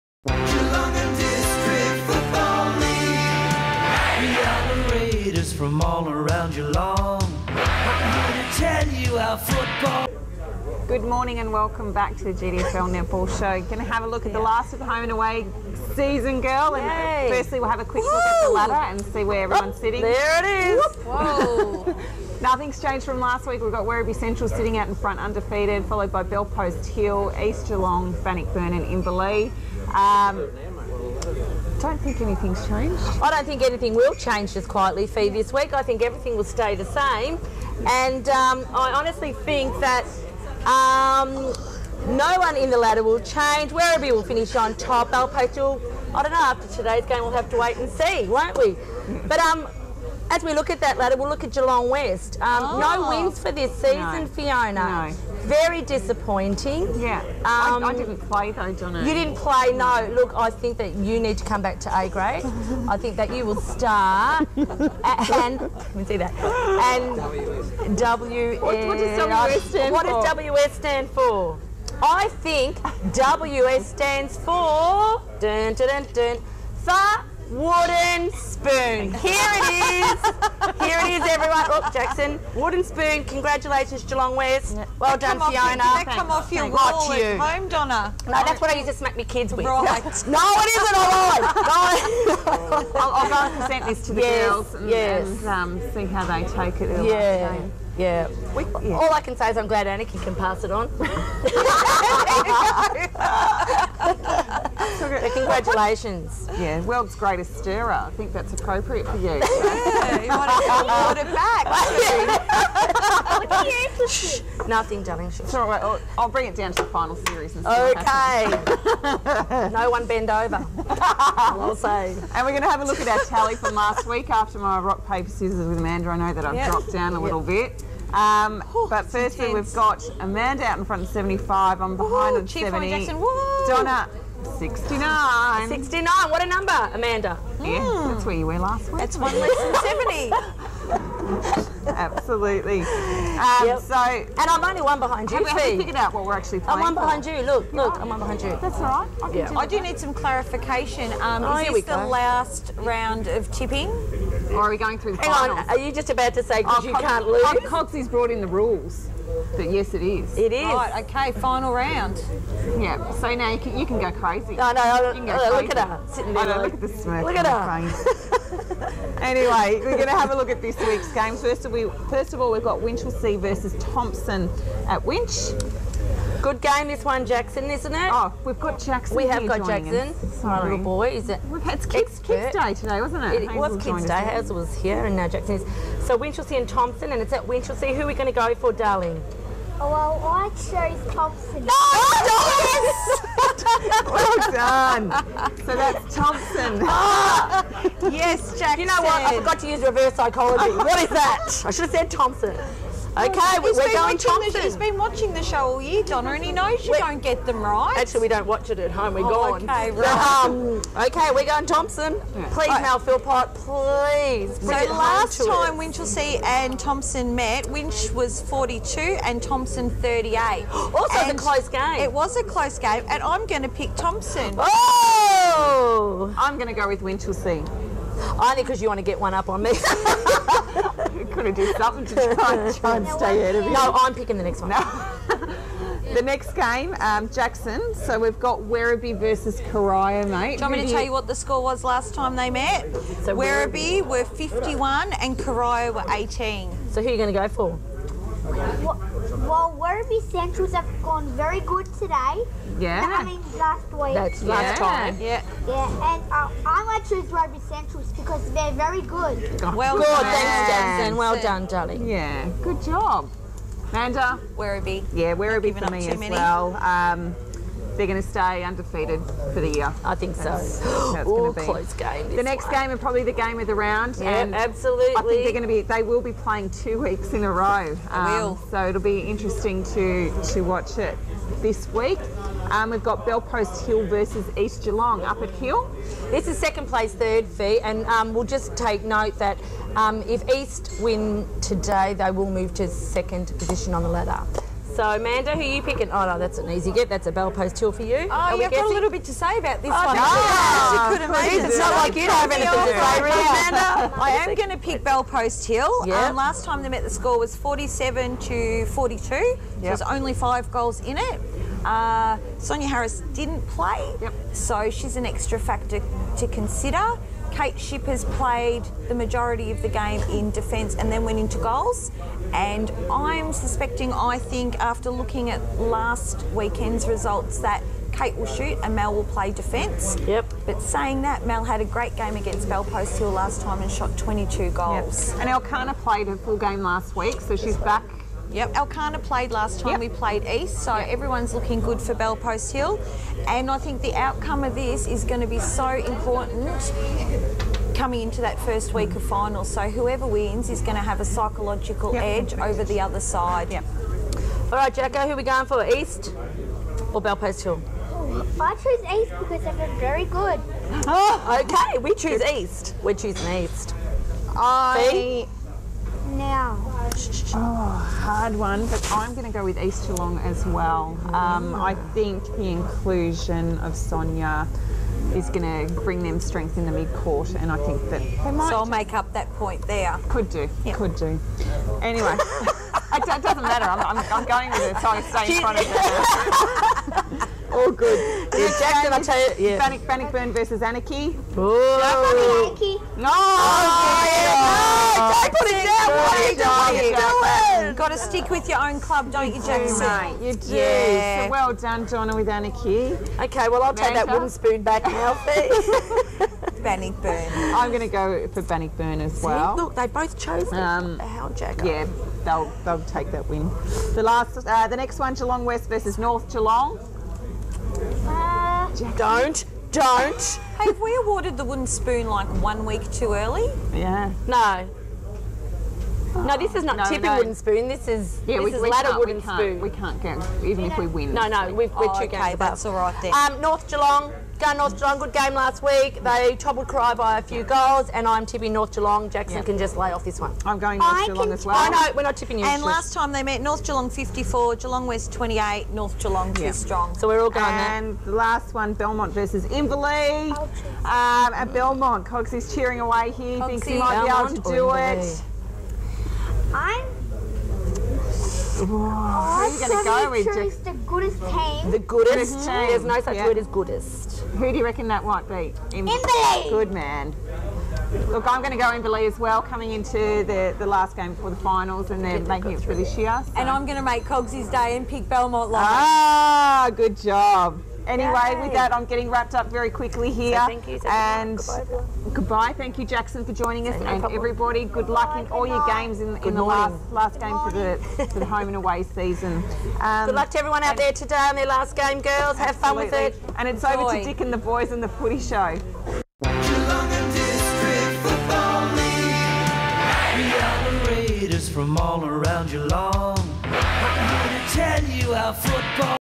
Pt3 GDFL Netball No20 17-8-13
Hosted by Fiona Neil and with Amanda Handley and Donna Francese the girls present the Geelong and District Football netnall news, views and interviews from around the League Pt3